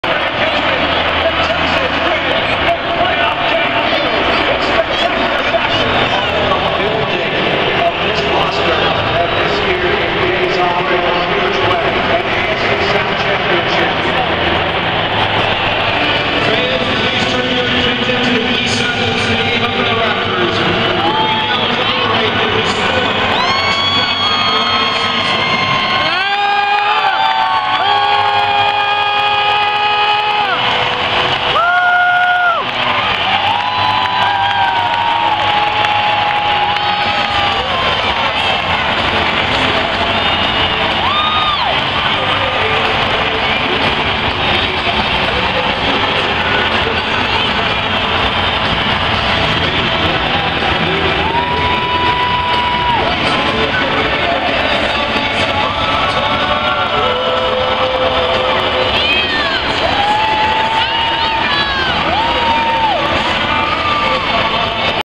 Texans 2011 division winning banner
This was taken when it was dropped before the Miami game..